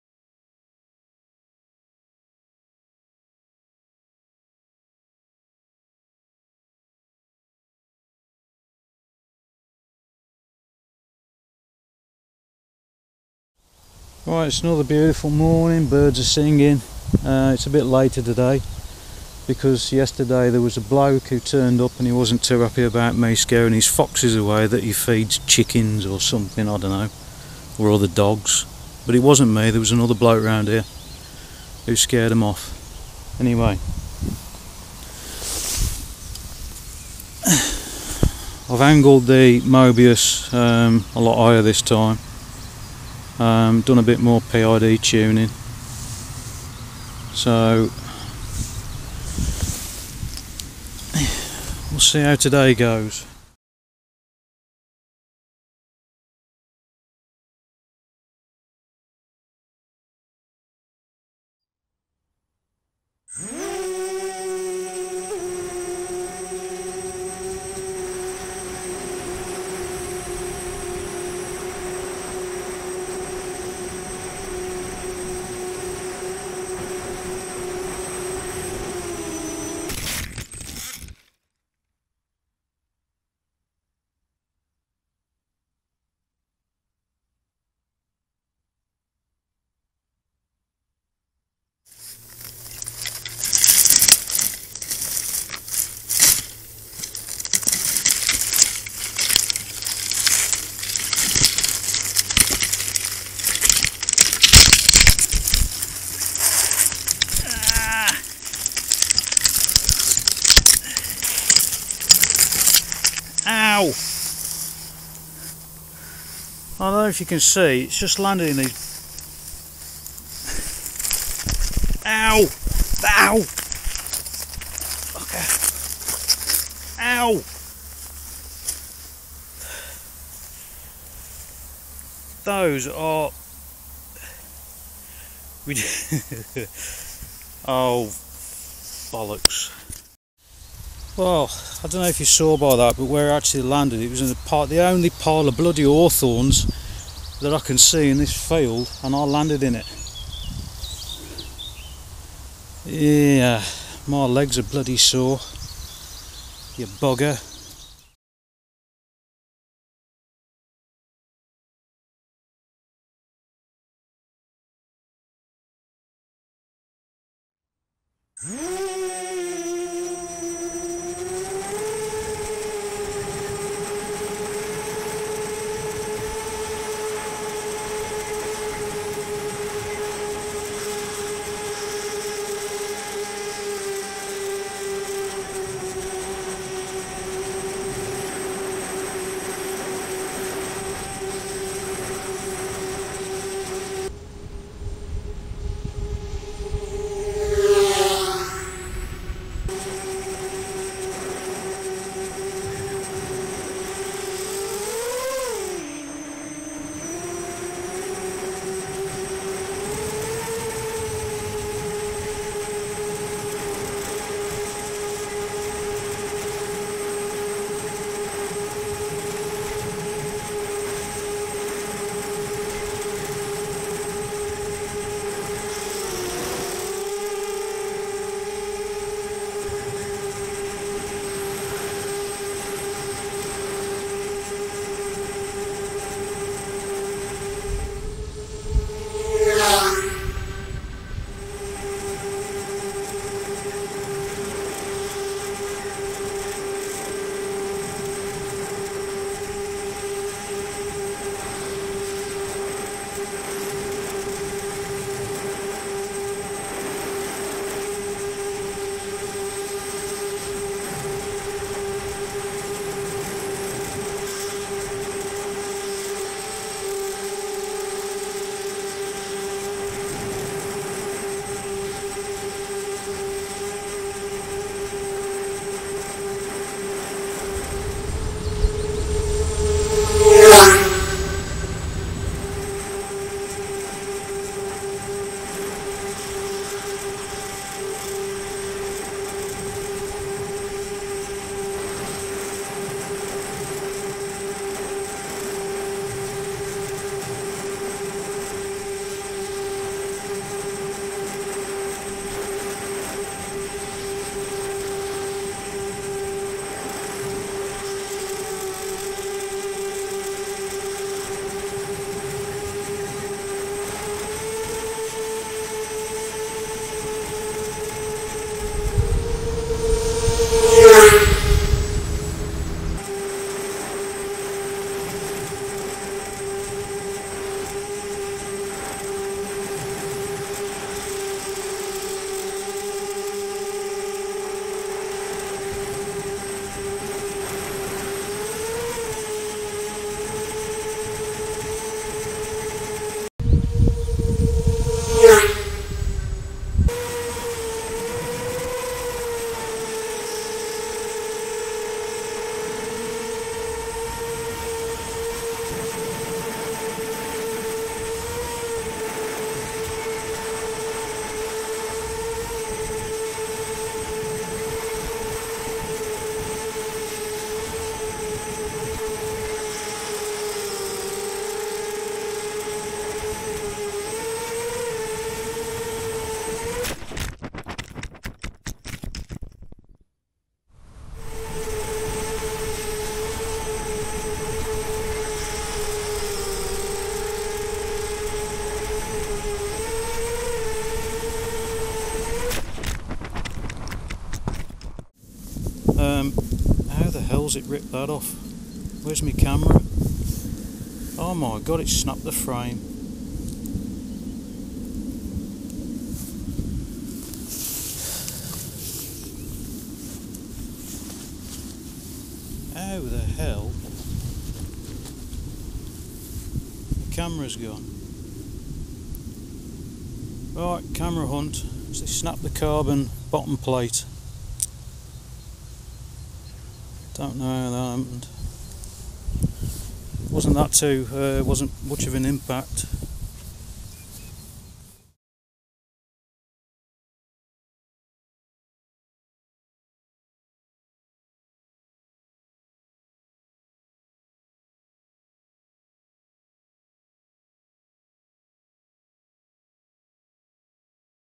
250mm mini quad - Naze32 - It's crash time again!
I dropped the RC rate a little and I think I forgot. It all happened so quickly :)
The first crash was possibly caused by my stupidity but the second crash was definitely caused by my stupidity.
The quad has been rebuilt, I've restored the Naze settings to how they were yesterday and I've fitted Tiger MN2206 and 6x5 props (cut down from 9x5) to see if the Gemfan 6045 props were causing the 'crabbing' issues.